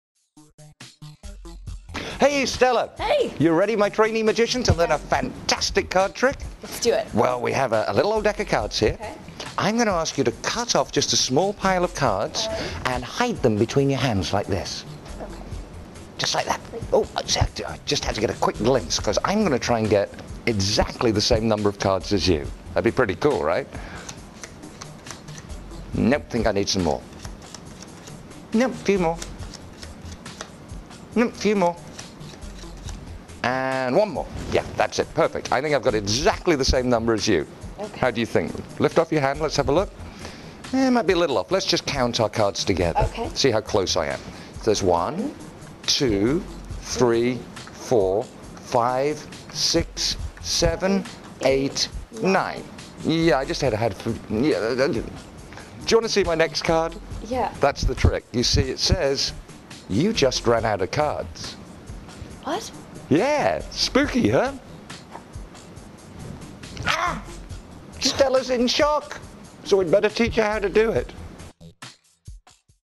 Fantasma Magic Moments - You Just Ran Out of Cards
Fantasma Magician Simon Lovell performs his new trick, "You Just Ran Out of Cards." To see how this trick is done, and for more like it,